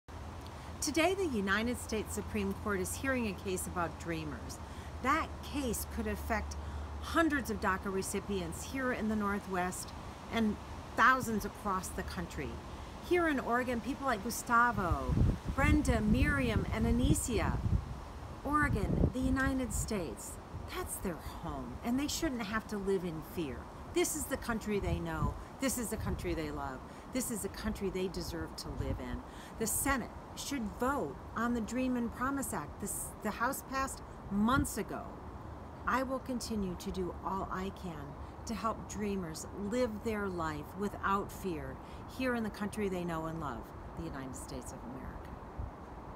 191112 DACA SCOTUS video
Congresswoman Bonamici stands with Dreamers